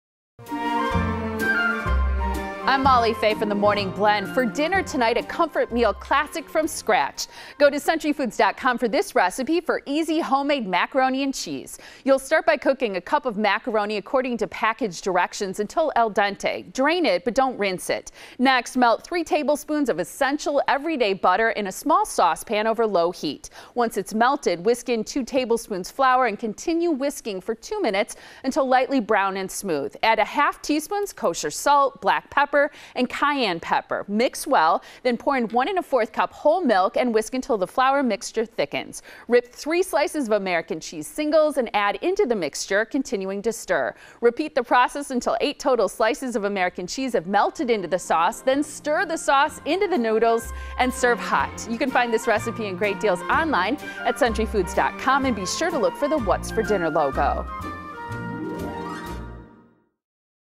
What's for Dinner? - Easy Homemade Macaroni & Cheese
We answer that age-old question... What's for Dinner? This week, we have a classic comfort food that the whole family will love! It's Easy Homemade Macaroni & Cheese. For more information and this complete recipe, check out SentryFoods.com.